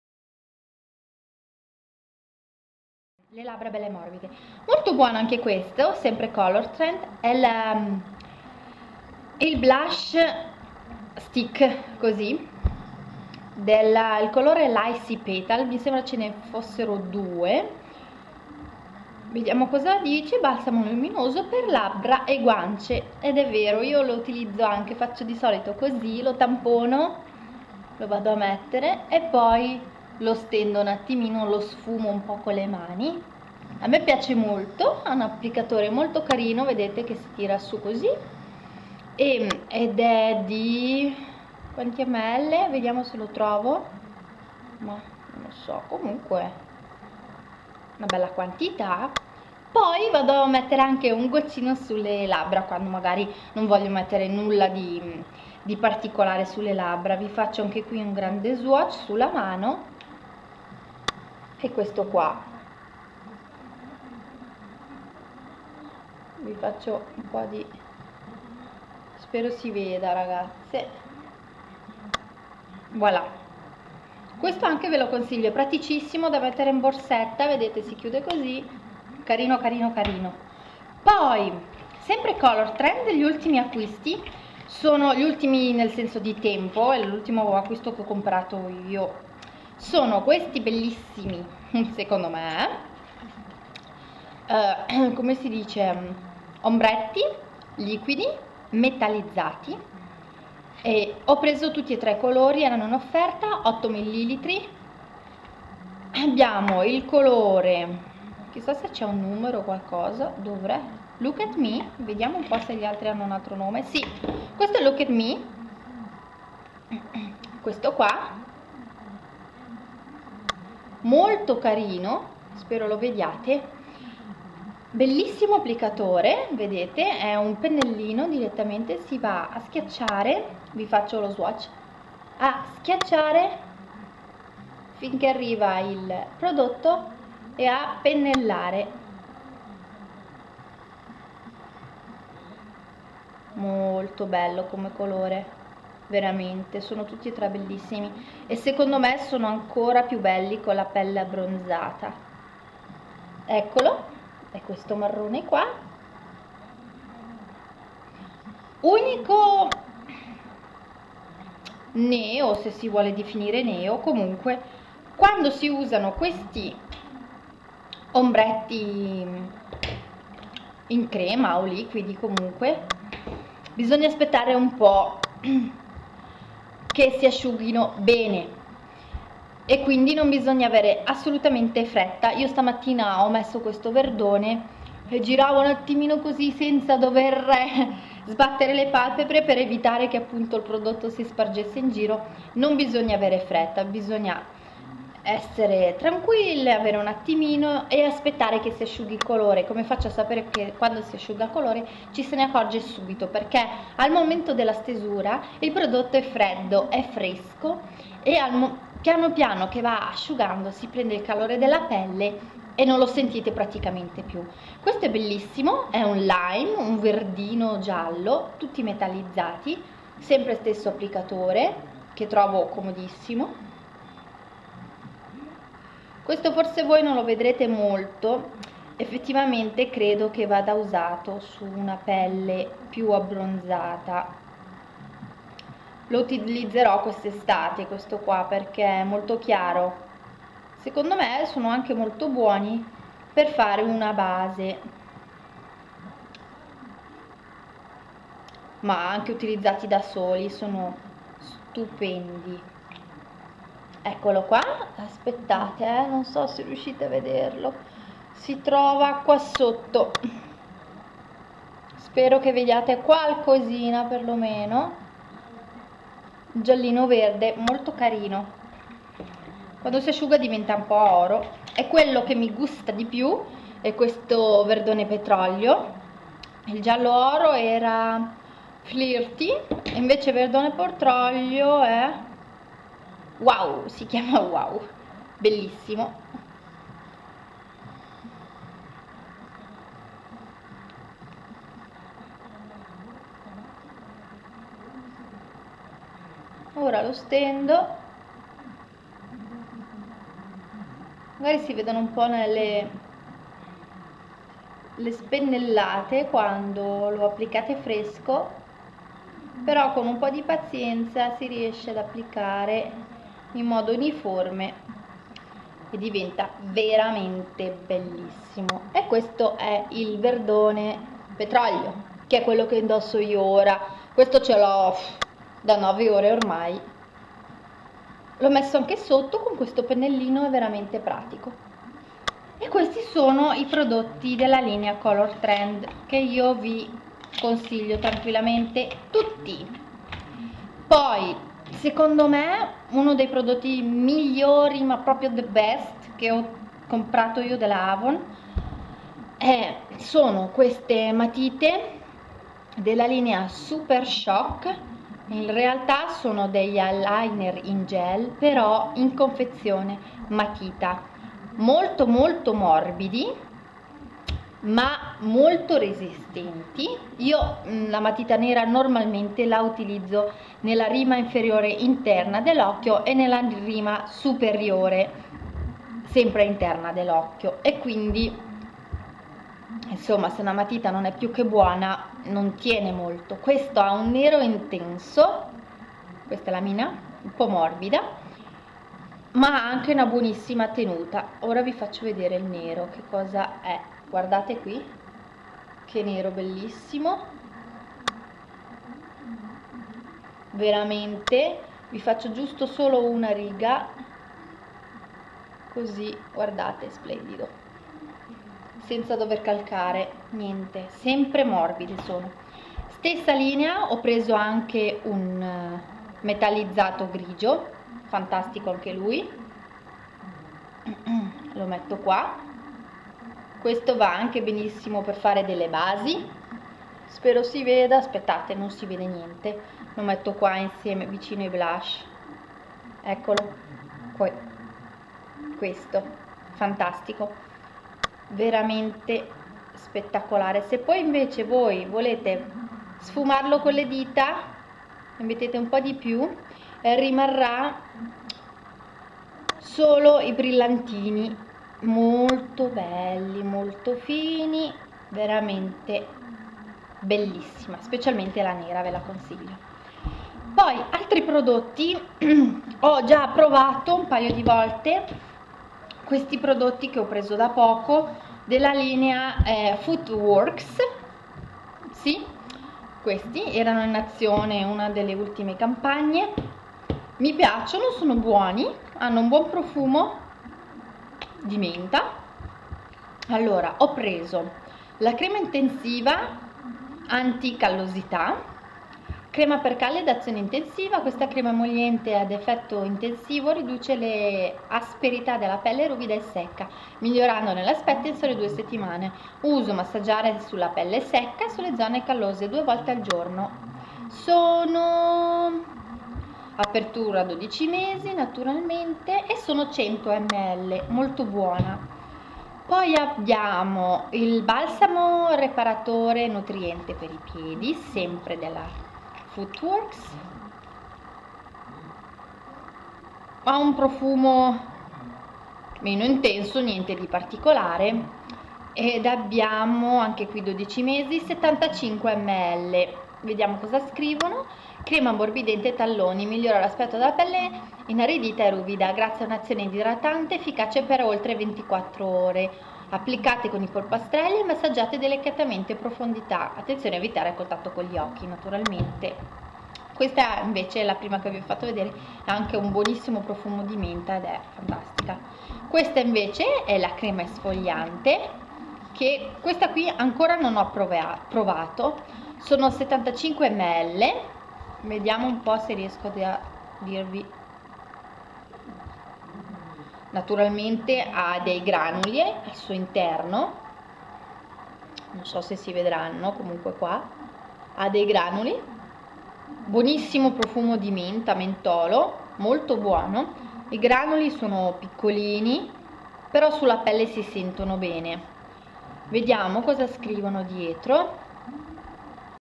REVIEW AVON -2A PARTE -
Le mie impressioni su qualche prodotto AVON acquistati nelle ultime campagne.
Per i prodotti che ho trovato nella campagna Nr. 18 (attuale) vi ho messo i prezzi.